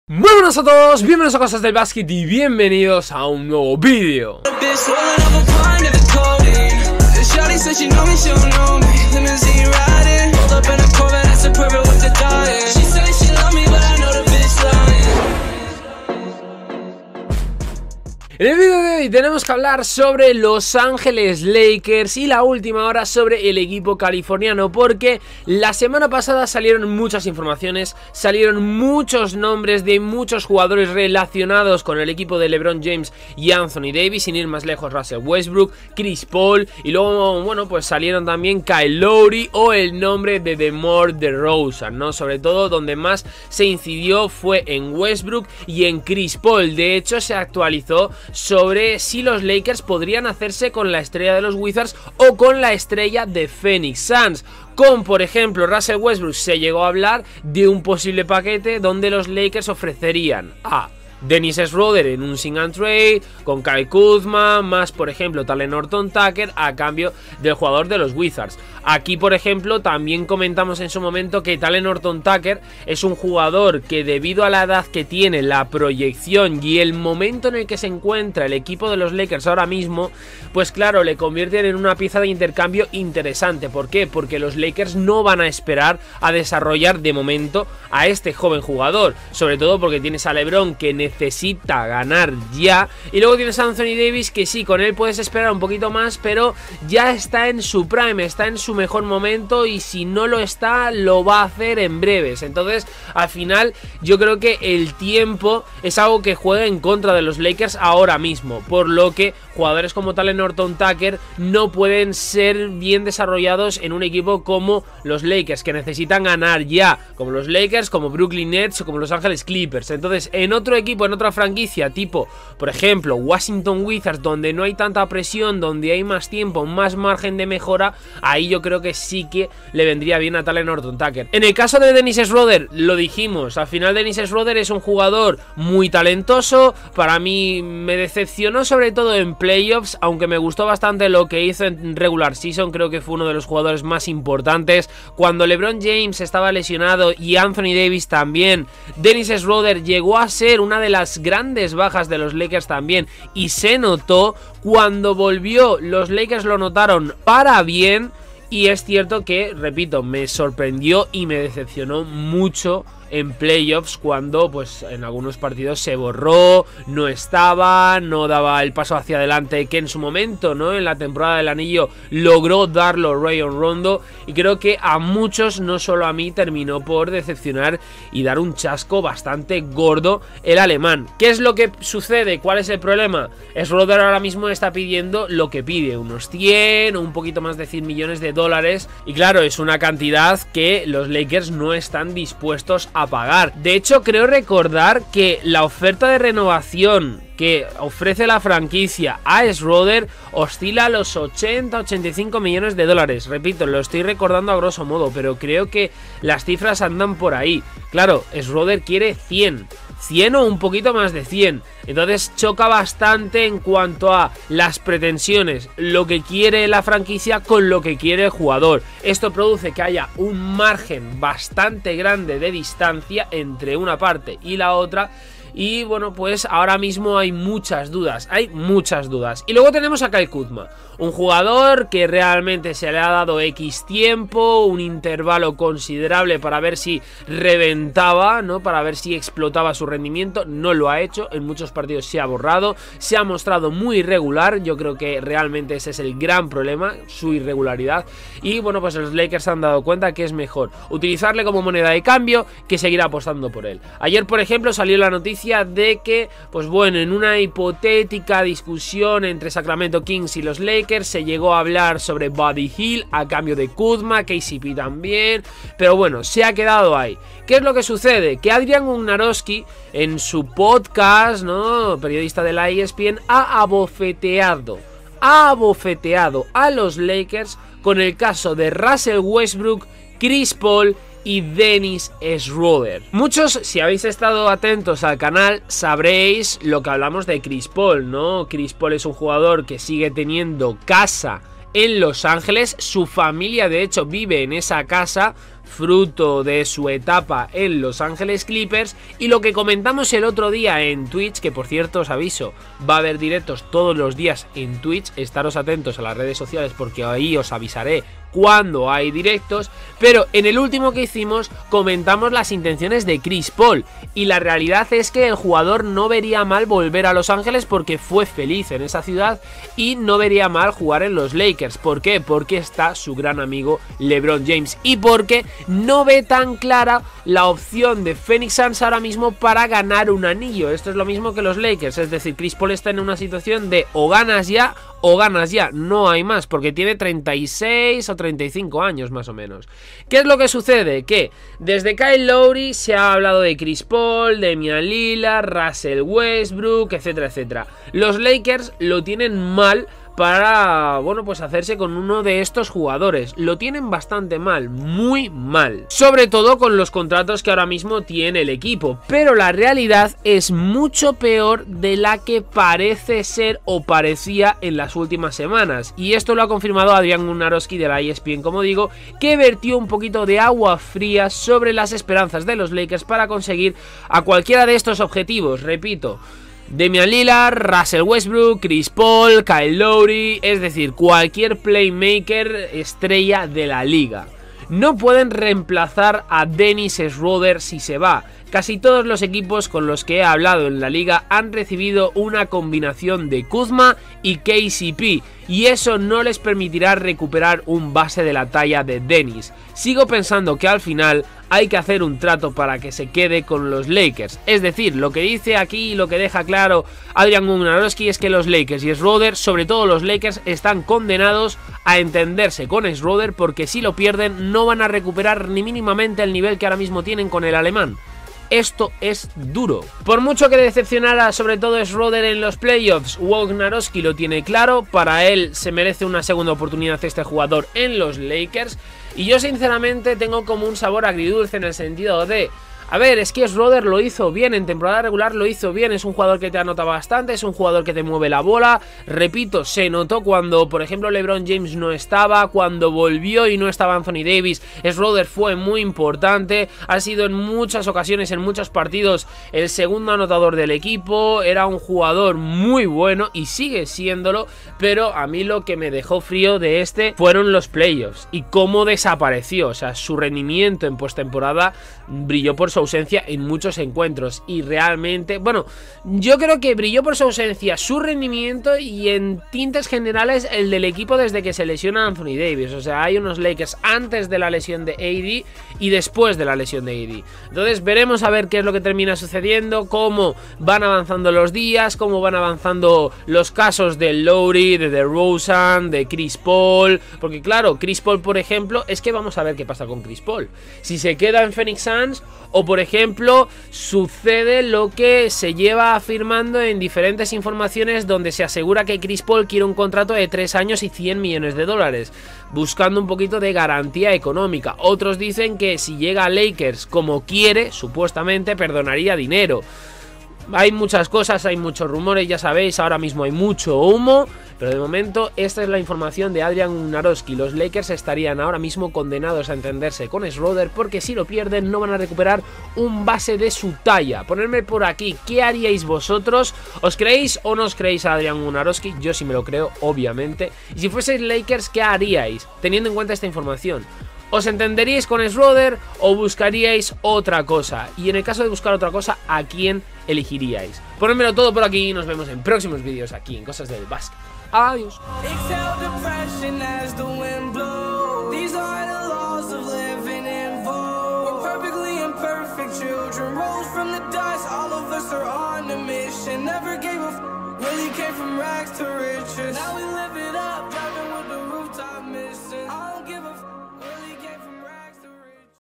💥WOJ ABOFETEA A LAKERS Y LEBRON JAMES POR CHRIS PAUL Y WESTBROOK🤯
En el DIRECTO de hoy, comentaremos las últimas NOTICIAS NBA donde LOSANGELESLAKERS y LEBRONJAMES han recibido una mala noticia por parte de la ESPN y ADRIAN WOJNAROWSKI en relación a DENNIS SCHRÖDER, CHRIS PAUL, RUSSELL WESTBROOK, KYLE KUZMA o KYLE LOWRY sin olvidar el rumor sobre un traspaso con SACRAMENTO KINGS por BUDDY HIELD.